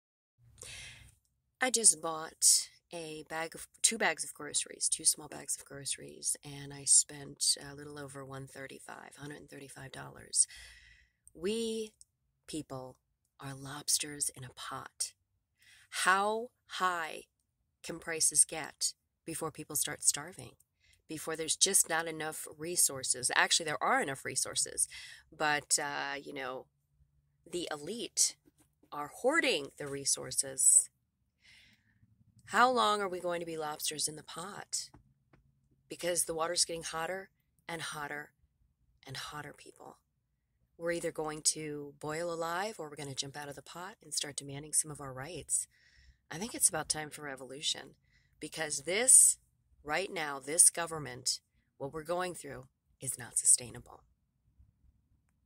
Like Lobsters in the Pot Are We!
Like many Americans, I don't know how we'll survive if prices for rent, mortgage, grocery and gas stay on the upwards trajectory they are currently on.

The poor working class have become the proverbial lobsters in the pot. The powers that currently be have been SLOWLY increasing the heat over decades. When the water finally hits it's boiling point, it will be far too late to do anything about it. This current fascist oligarchy will destroy us from the inside out.

Freedoms will be lost, forever.

Government protections will start looking like iron shackles and rusty chains.

The working class will become the new financial slaves to a heartless capitalist regime, trapped by bad credit scores, obscenely high interest rates, late fees, taxes, housing costs...the list goes on and on.

REVOLUTION, people...if every American citizen decided to forgo paying their taxes for just one year, we would have this government over a barrel.

Remember to start where you are, see what needs tending, and do what you can!

Namaste ❤